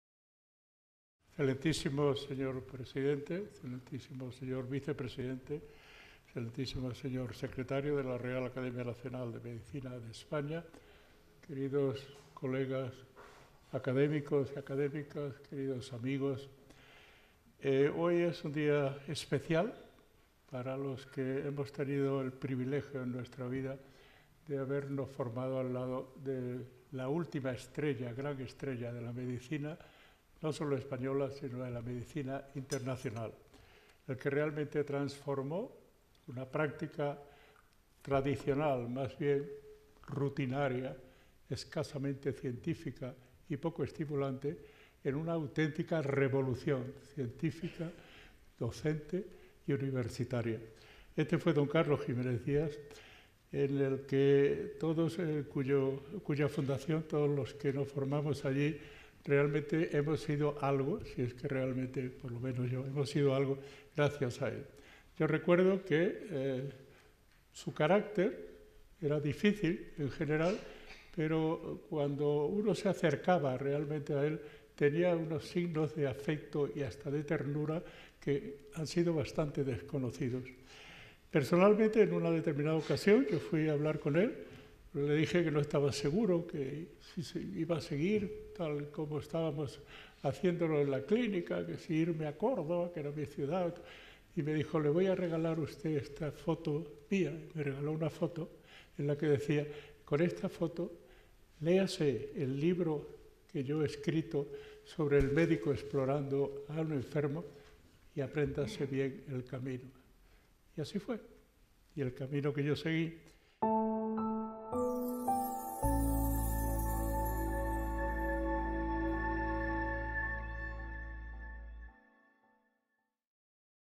El Profesor Jiménez Díaz pionero español de la Medicina Integral · 10 de abril de 2018
Link para ver el vídeo completo en Real Academia Nacional de Medicina de España Televisión (ranm.tv)

EL PROFESOR JIMÉNEZ DÍAZ PIONERO ESPAÑOL DE LA MEDICINA INTEGRAL (CLÍNICA, INVESTIGACIÓN Y DOCENCIA)

Prof. Manuel Serrano Ríos

Académico de Número de la RANM de España